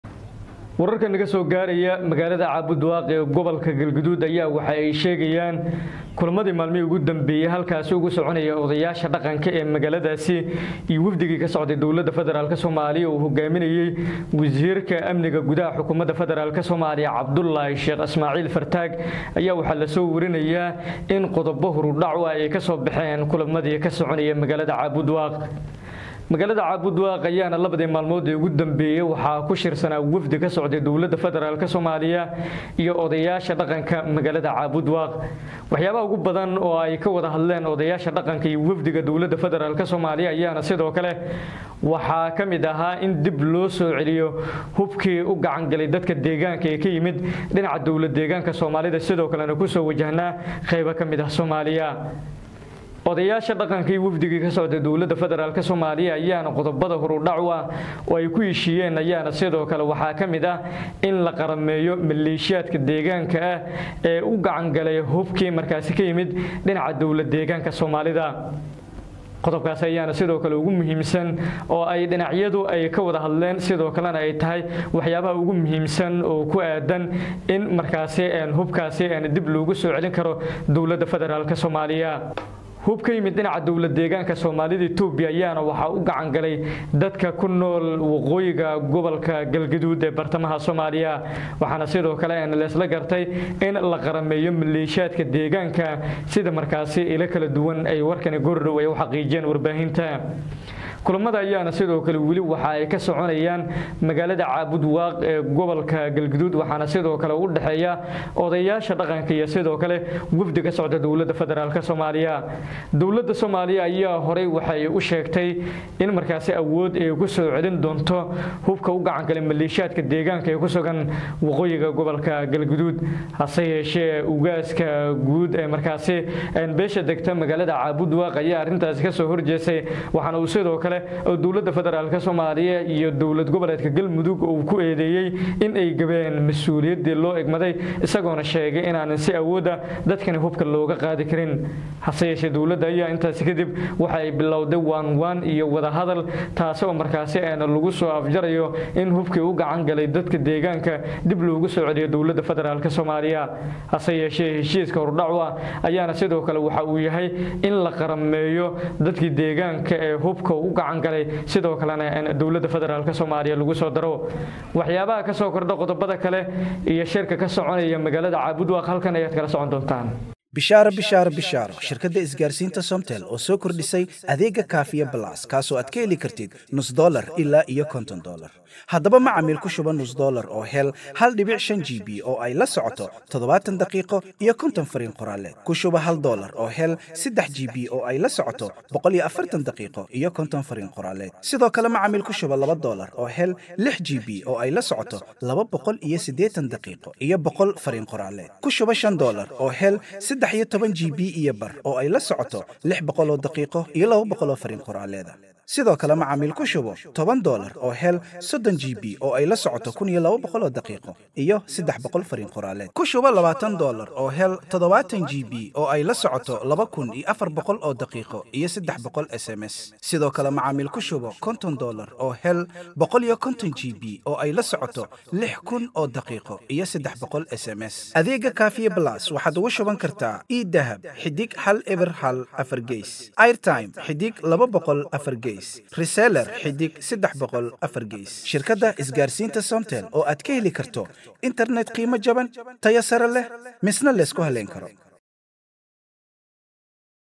Muxuu yahay Heshiiska Laga Gaaray Hubkii dhawaan Lagu boobay Cabudwaaq.?
Dowladda Soomaaliya iyo Odayasha dhaqanka Caabud waaq oo Hishiis ka Gaaray Hubki Shacabka u Gacan Galay.